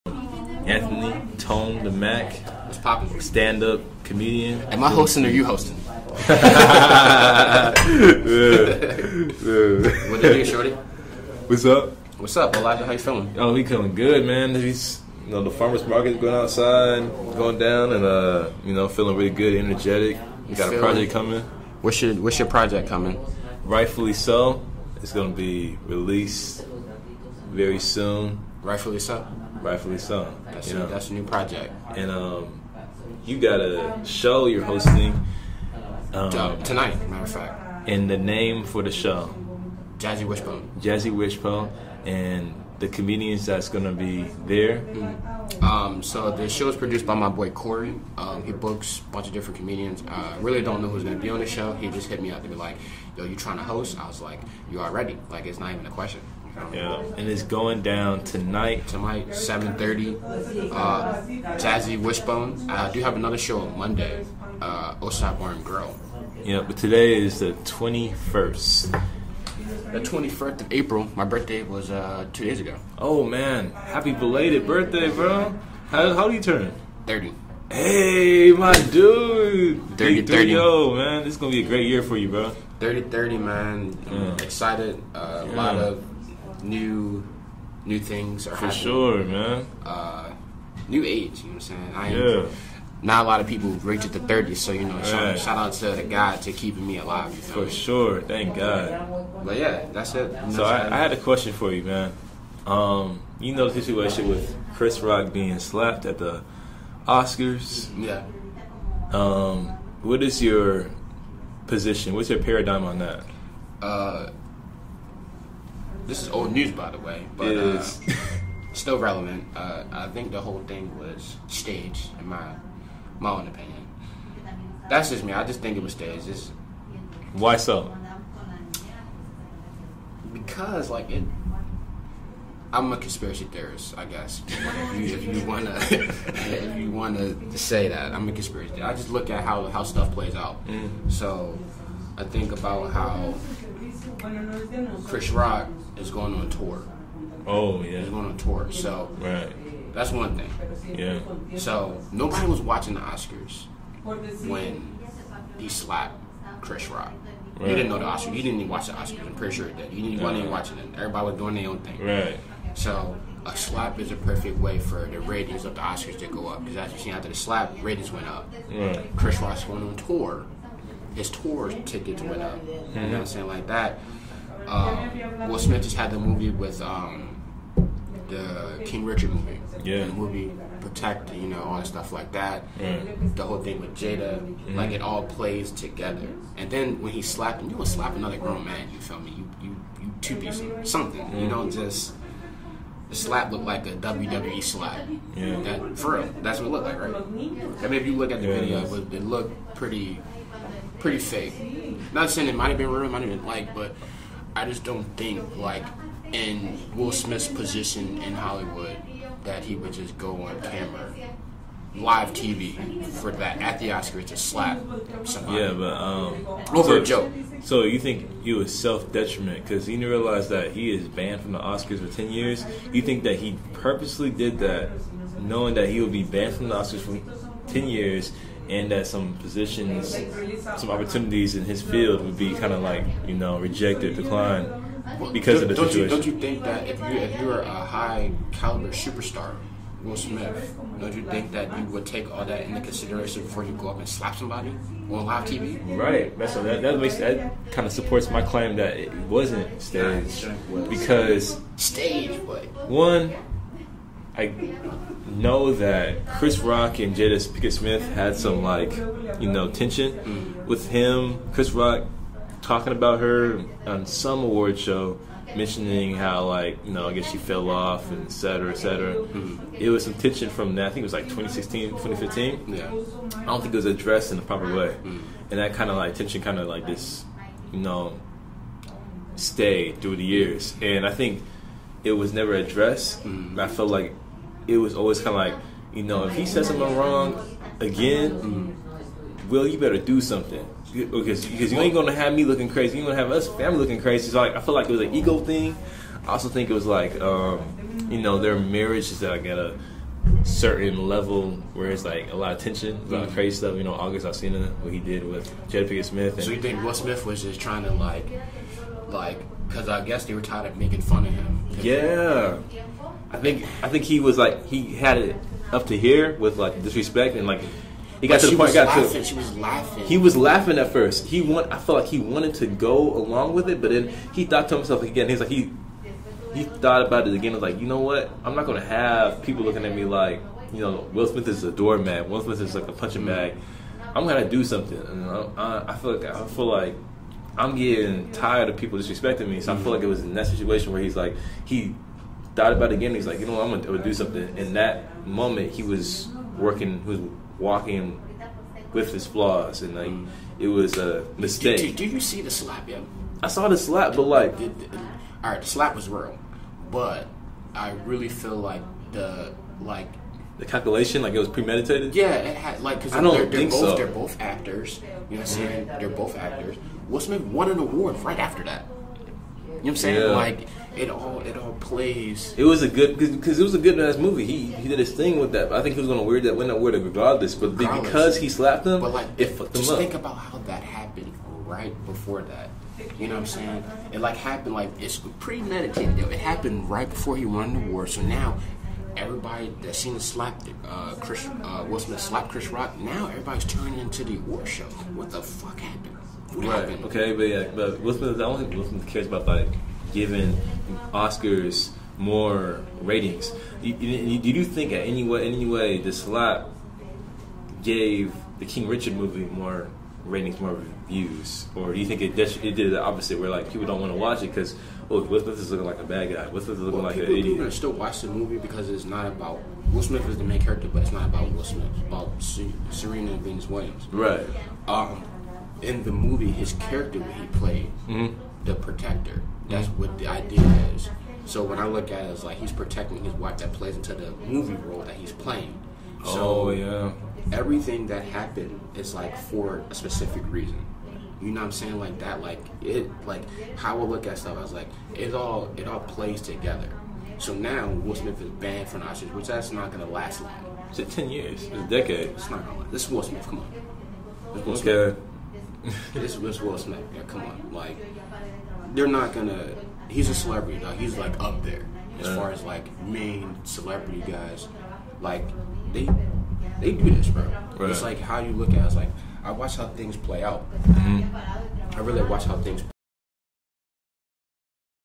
“You’re Gonna Be Ok” Podcast Episode 47 “Will It Slap” By: Tonedmack ft. Olijah
Comedian, “Tonedmack” and rapper Olijah discuss their views on Will Smith slapping Chris Rock at the 2022 Oscars, Bel Air, Jack Harlow and Kendrick Lamar’s album dropping the same month of May and More!